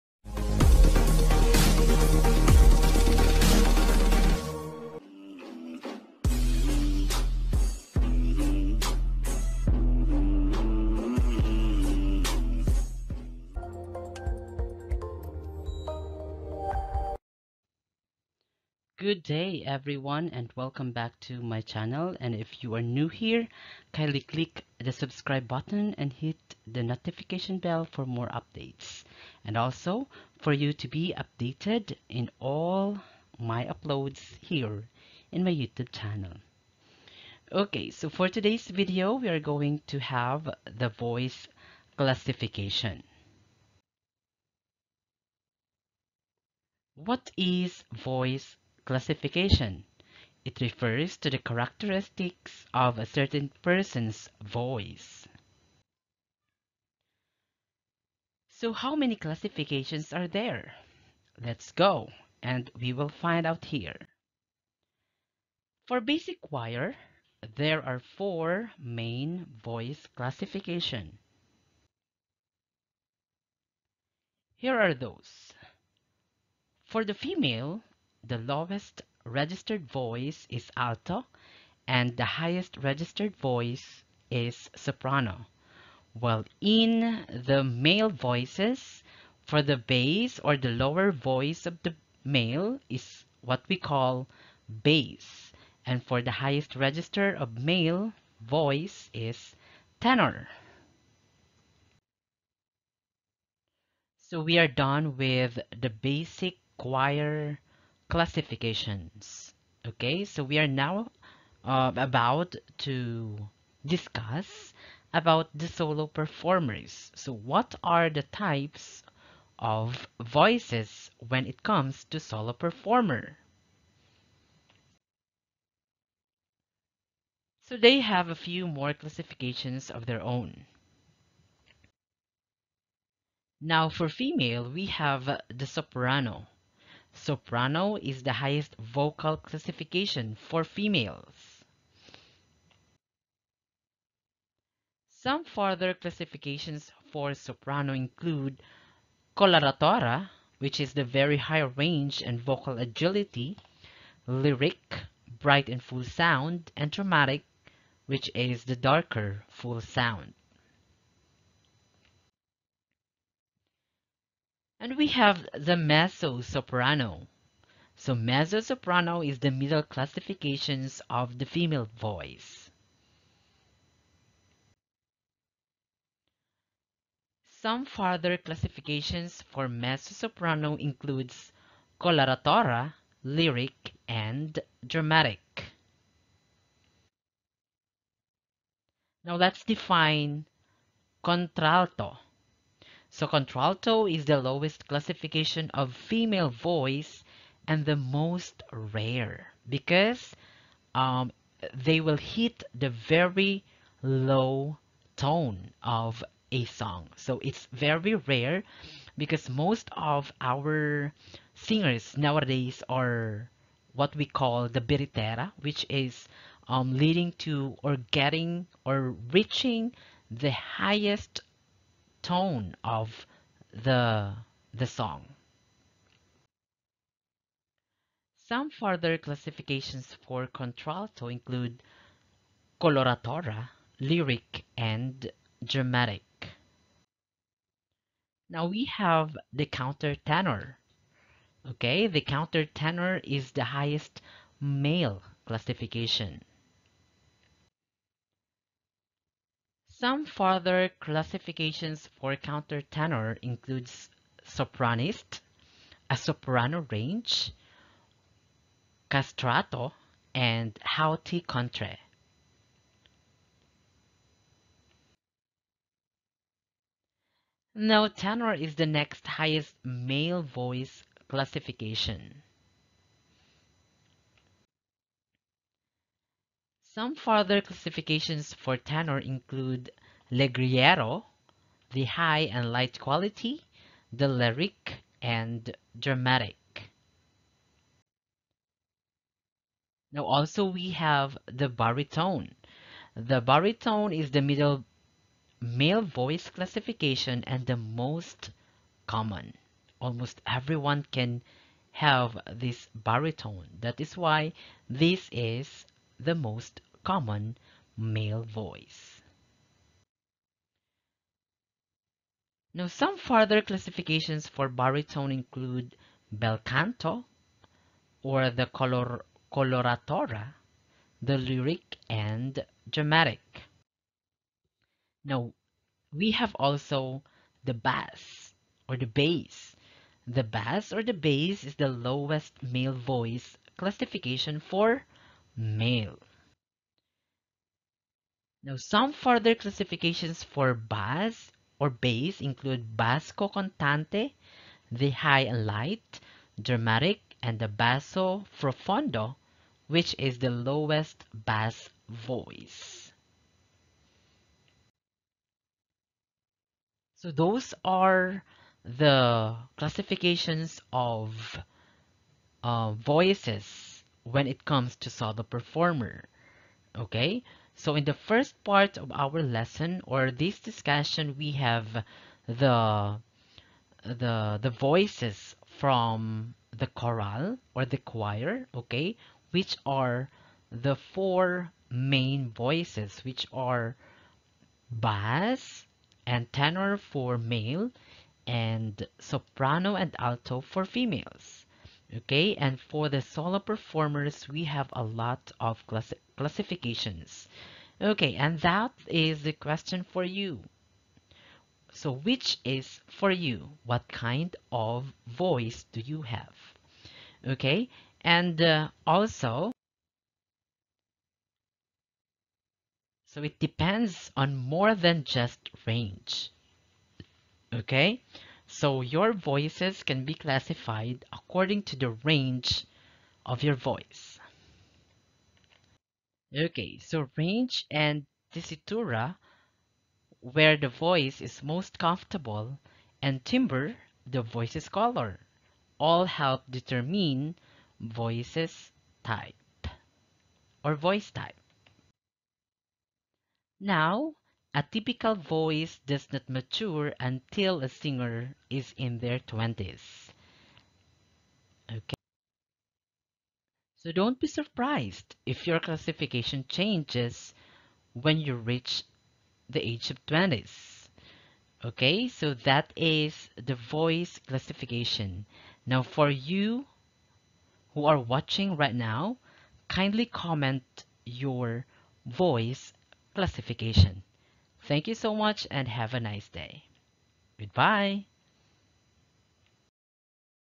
Voice Classification or Types of Voices
This video will tell you what kind of voice do you have. Come and join me as we know the other voice types.